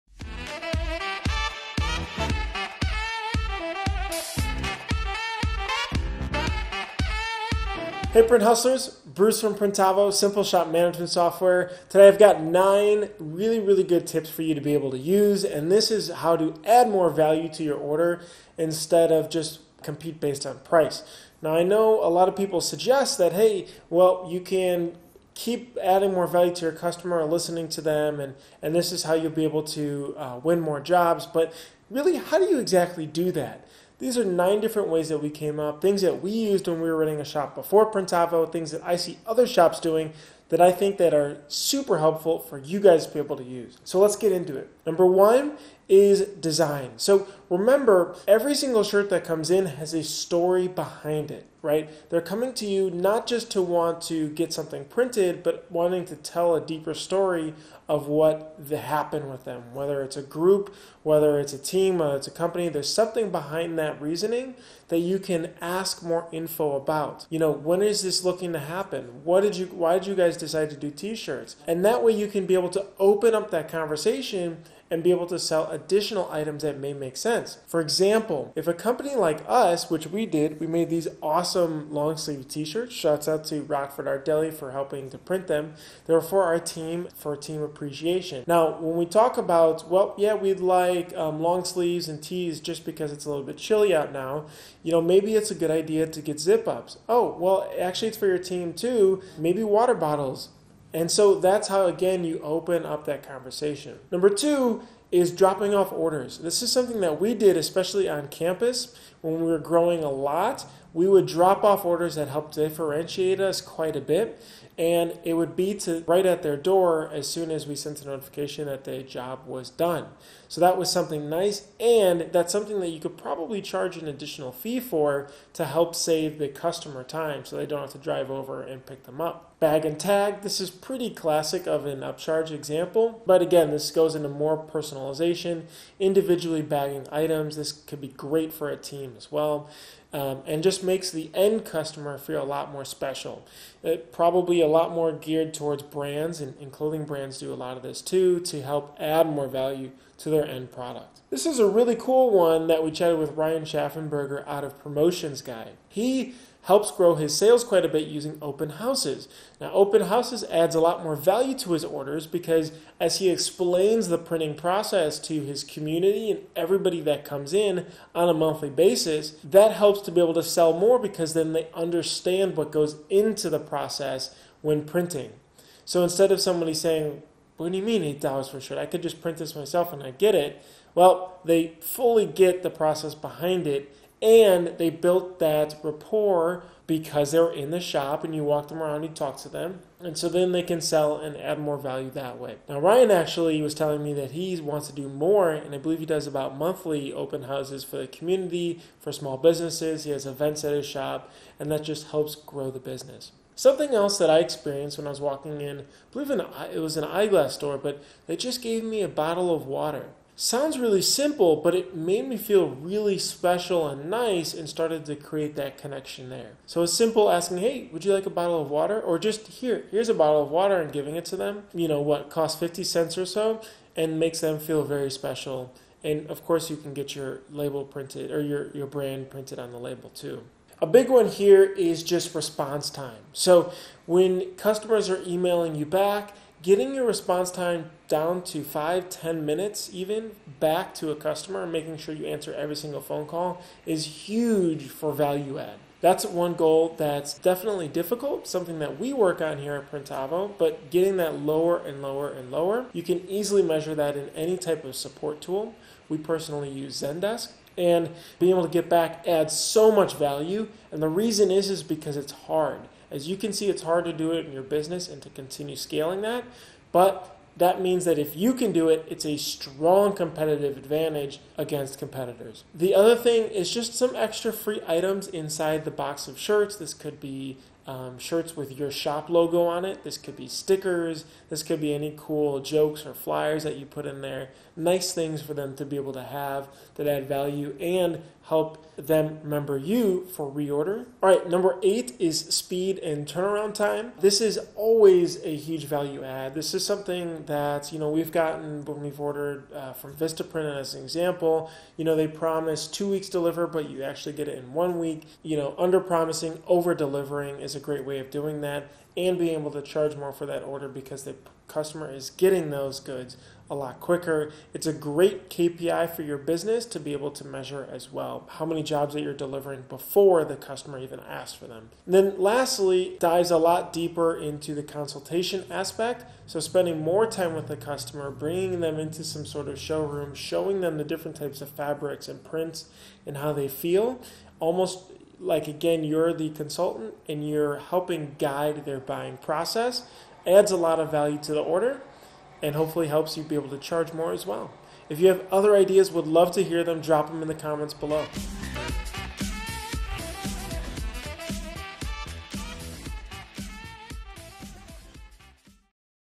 9 Ways to Compete Other Than Being the Cheapest
You don't want to be the cheapest print shop on the block. You can't sustain a profitable business that way. So how exactly do you add value to your custom printing orders?

1. Design for the story behind the shirt. Why did that group, school, team, or organization decide to get custom printed merchandise? That can lead your design and let you connect with the story.
2. Drop off orders. Delivering to their door for a small fee can really differentiate your shop.
3. Bag-and-tag. This makes the end customer feel a lot more special.
4. Open house events. When everyone comes to your shop, they understand the process behind screen printing. This connects you with the community.
5. Give customers a bottle of water. Sounds simple, but makes customers feel really special and nice. You could even get your brand printed on the label.
6. Get customer response times down. Something we work on at Printavo, getting back quickly adds value. It's hard to do! But it's a strong competitive advantage.
7. Freebies. Even small things like stickers, flyers, and jelly beans will help them remember your shop.
8. Turnaround time. Underpromising and overdelivering is a much better strategy. The fastest possible turnaround time is how to add powerful value. A great KPI (key performance indicator) for your business is how many orders are delivered ahead of schedule.
9. Be a customer consultant. Use your knowledge and expertise to strategically guide customers toward profitable prints and garments.